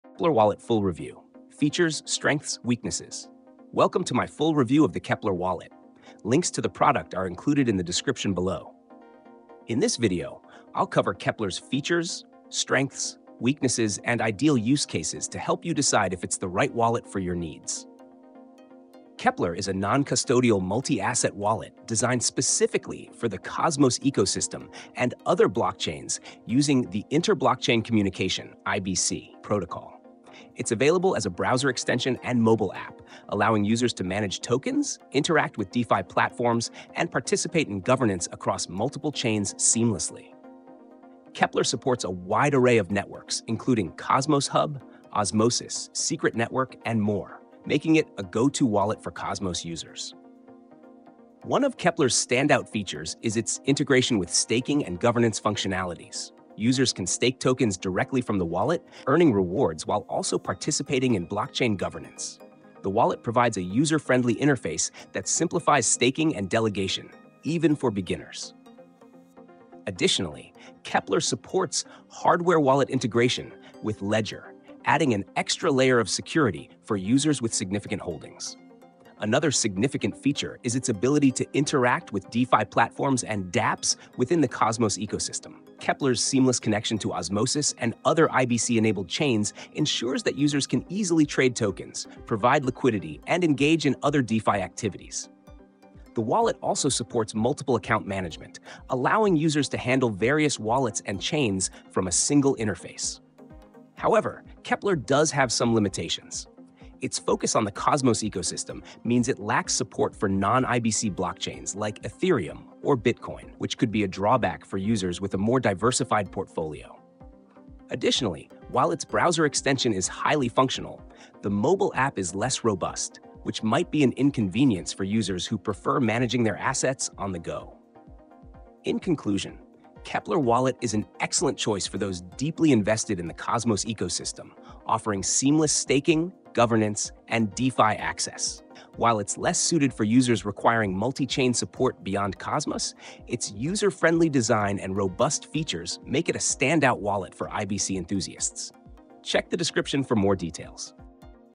Keplr Wallet Full Review - Features, Strengths, Weaknesses
In this video: Keplr Wallet Full Review - Features, Strengths, Weaknesses

I am 1st Knowledge Agent  on YouTube, hope, you will find all information you want to get